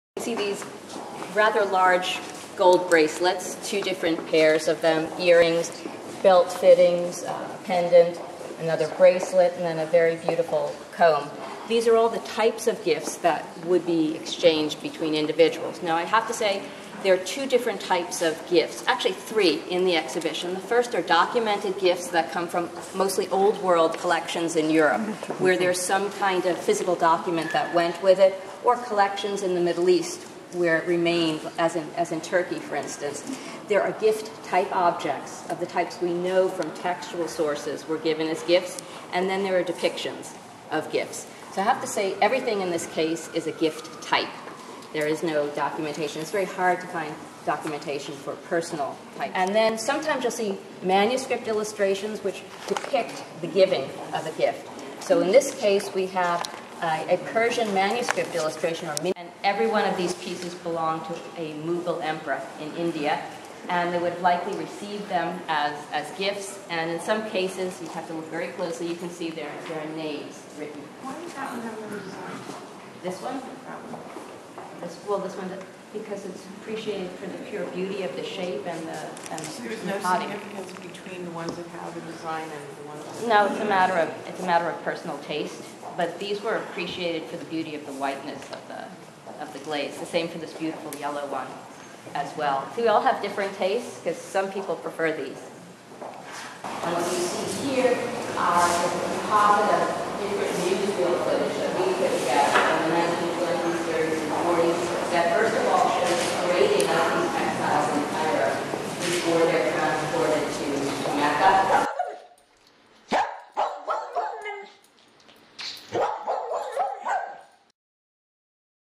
LACMA Gifts of Sultan Exhibition in 3D
In anaglyph 3D, Gifts of Sultan exhibition June 5 - September 5, 2011 with Linda Komaroff, curator, Islamc Art and Department Head, Art of the Middle East, Los Angeles County Museum of Art.